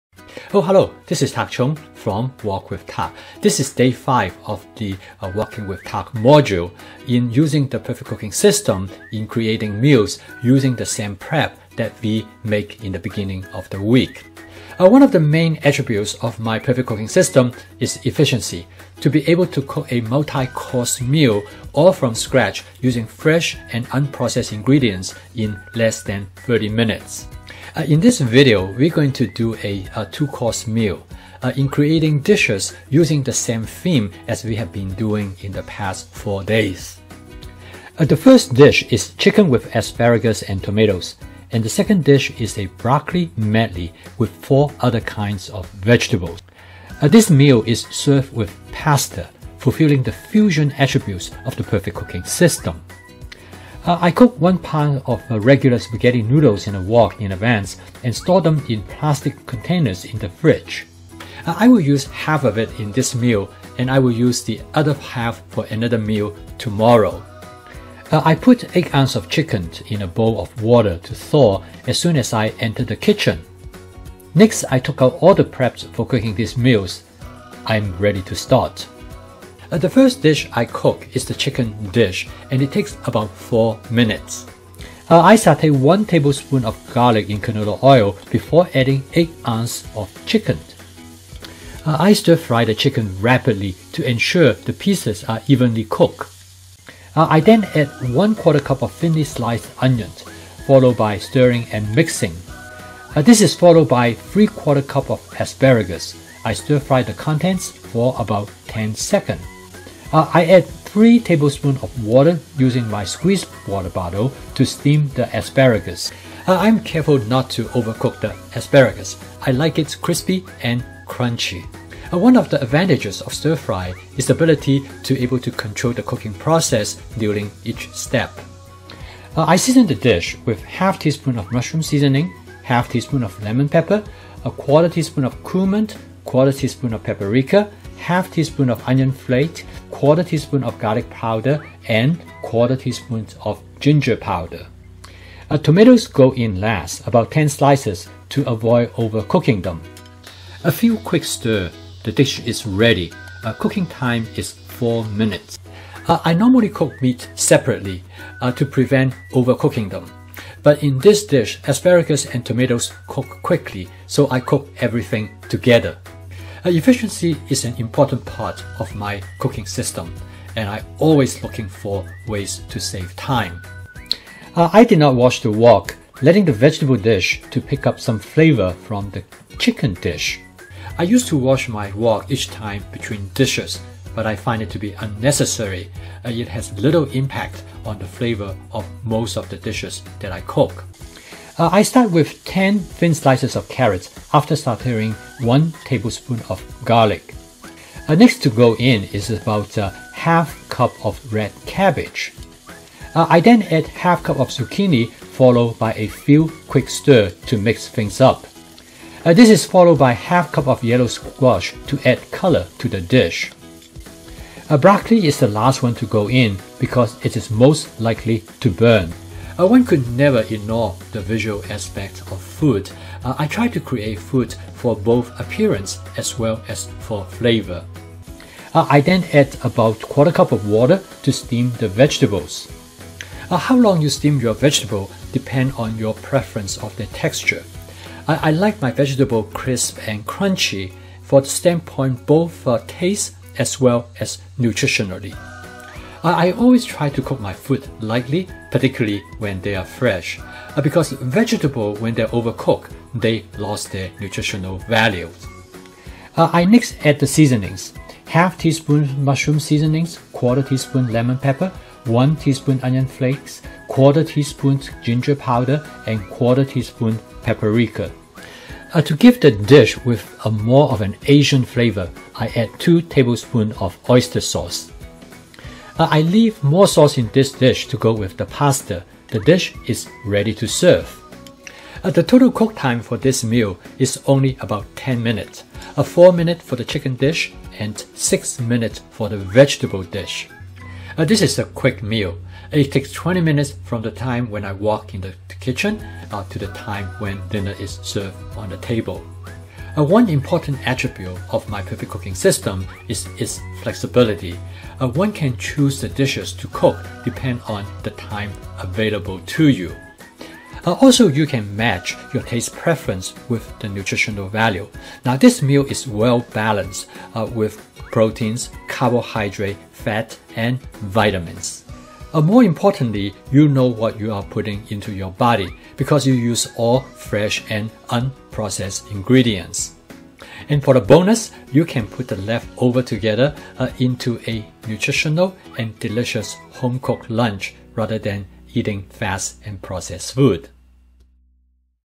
Day 5. Fast two-course meal from scratch under 20 minutes with the PERFECT Cooking System
This two-course meal is fast, taking only 10 minutes to cook two dishes, a chicken with asparagus dish and a broccoli vegetable medley dish.  This video is to demonstrate the flexibility of the cooking system to create different dishes according to the amount of time available.  The entire meal from the time entering the kitchen to the time meal being served is less than 20 min, all from scratch using fresh and unprocessed ingredients.